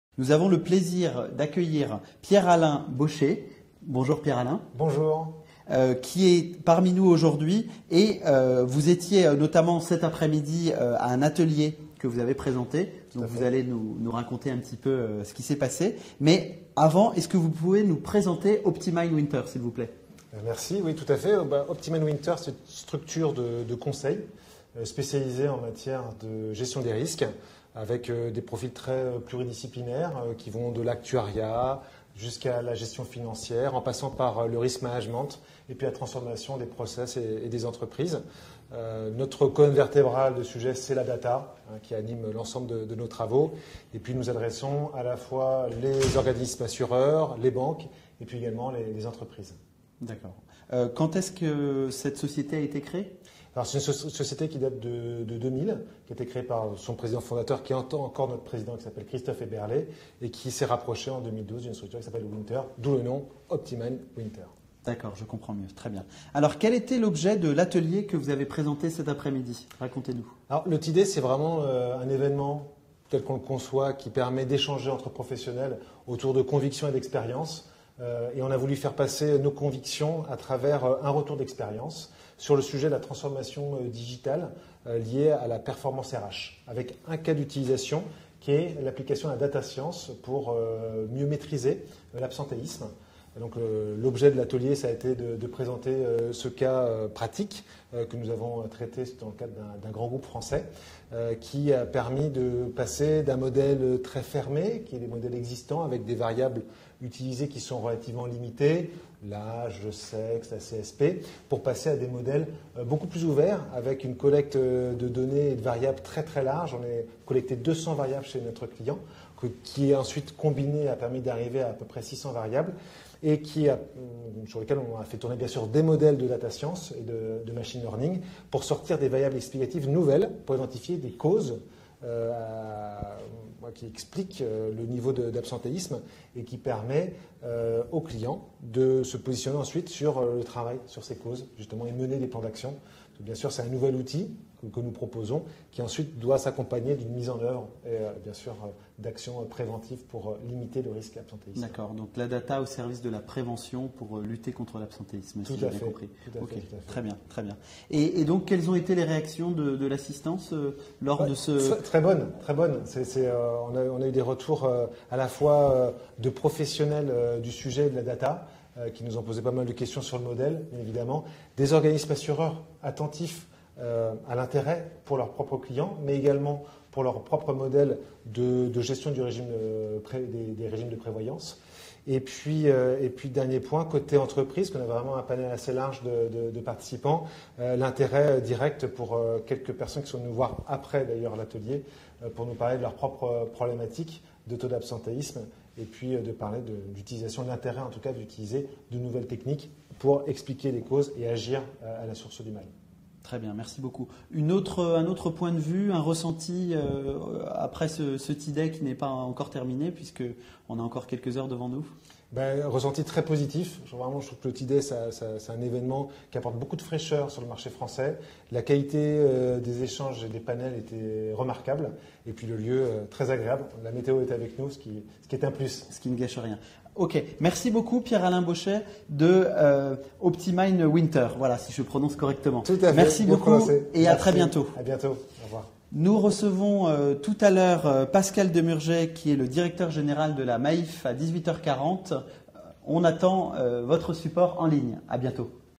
PIERRE-ALAIN BOSCHER - OPTIMIND WINTER
Enregistrement vidéo du Periscope Live de Pierre-Alain BOSCHER effectué lors du TDAY Insurance du 7 et 8 Juin 2017, organisé par Emmanuelle Lambijou et Jean-Luc Gambey.